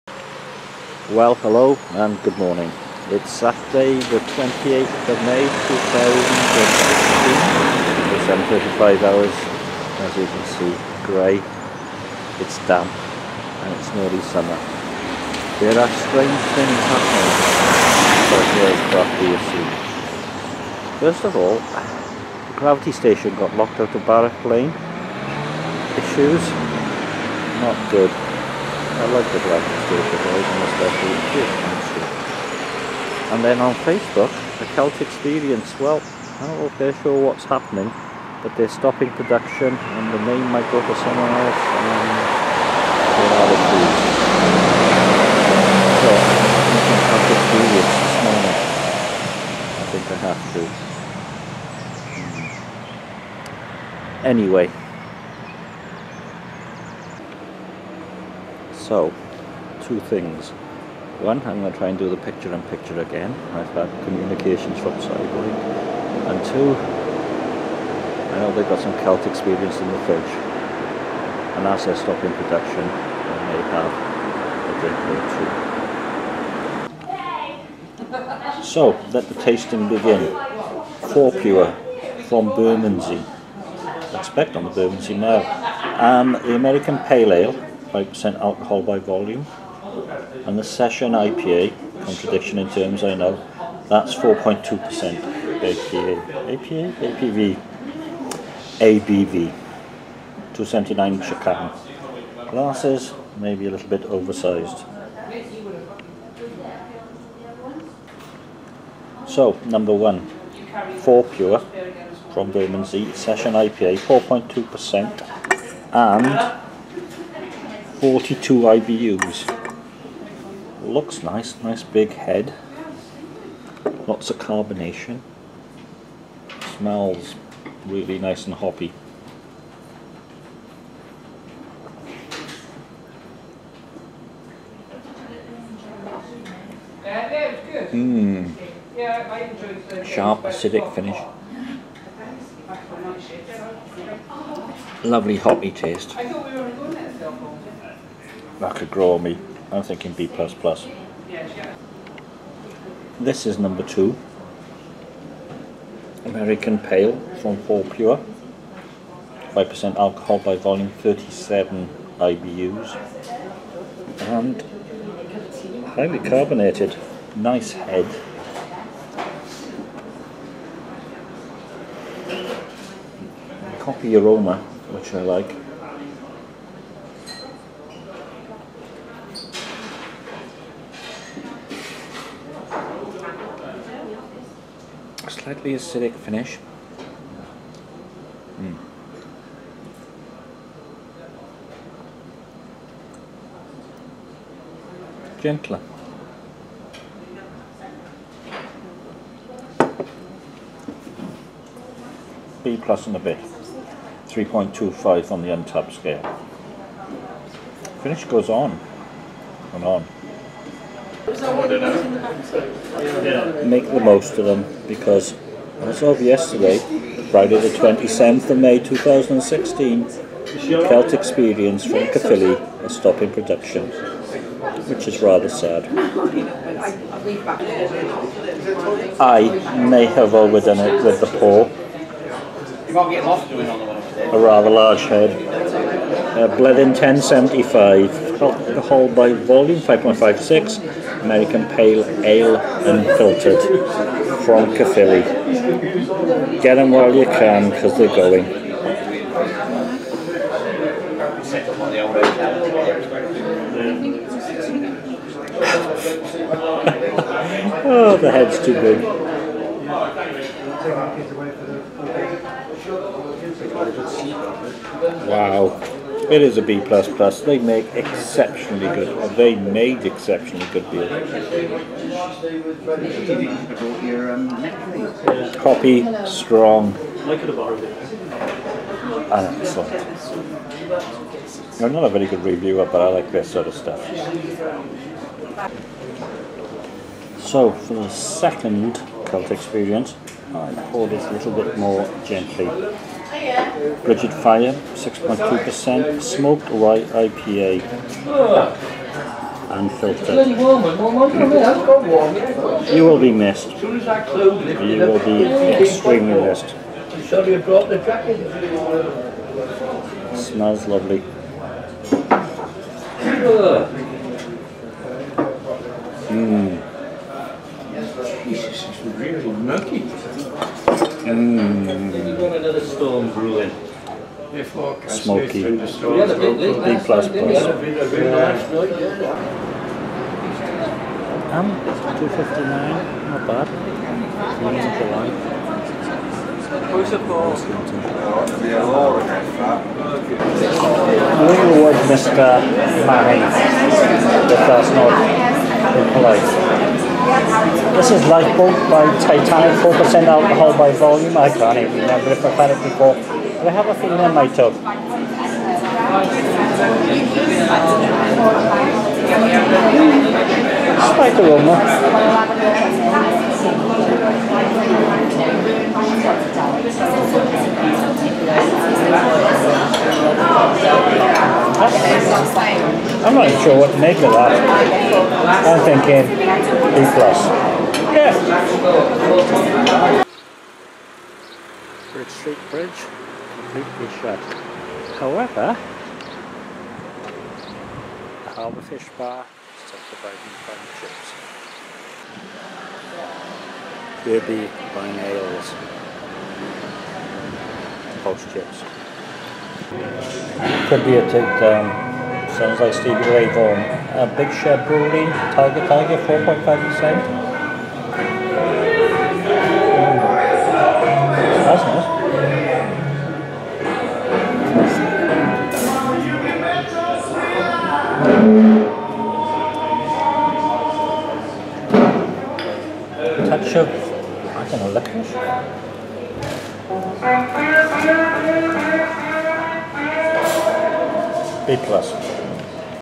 Sadly Ceasing
The full version of Saturday 28 May 2016. In Newport as there were more beers to be tasted in the Queens Hotel. Two lovely cans from Fourpure. Sadly, the day before The Celt Experience had announced it was ceasing production. So I had to have two bottles from them too. JWL, Harbour Fish and The Urban Tap House Newport also got visits. Almost Summer.